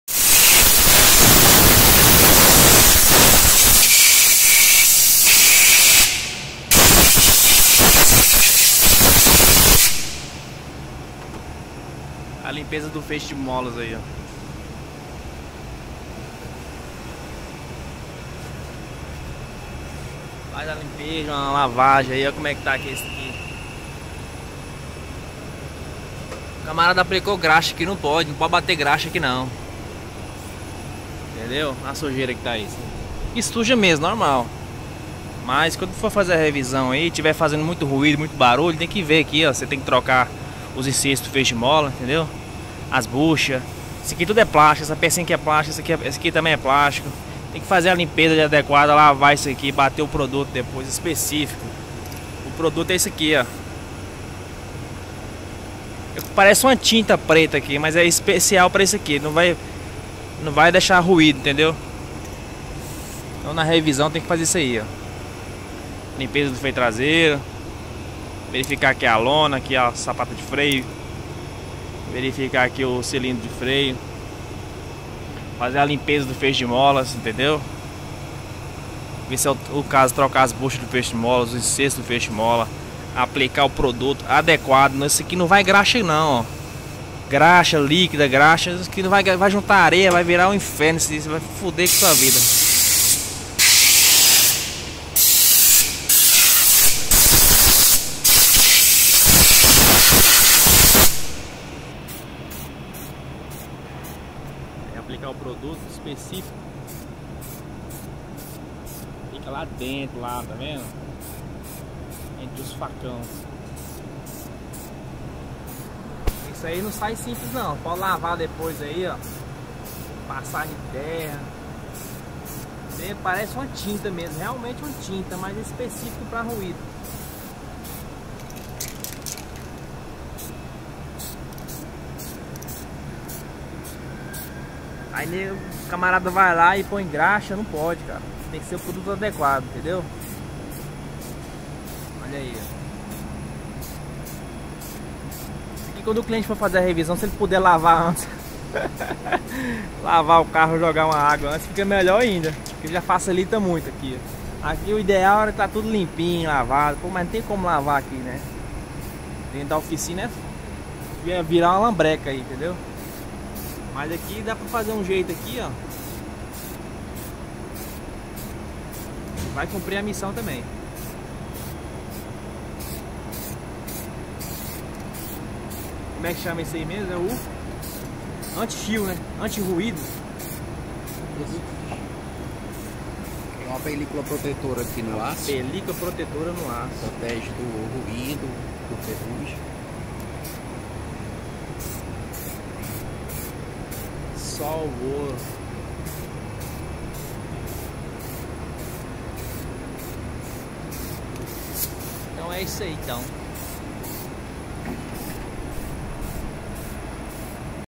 RANGIDO NA TRASEIRA DA CAMINHONETE ⚠️ LIMPEZA DO FEIXE DE MOLAS APLICAÇÃO DO PRODUTO ANTI RUIDO.
meus amigos
é sempre bom fazer a limpeza do feixe de molas...
ver se é nessecário fazer troca dos insertos do feixe de molas...
não pode aplicar graxa, Wd-40, desigripante,...
tem um  produto certo pra aplicar no feixe de molas.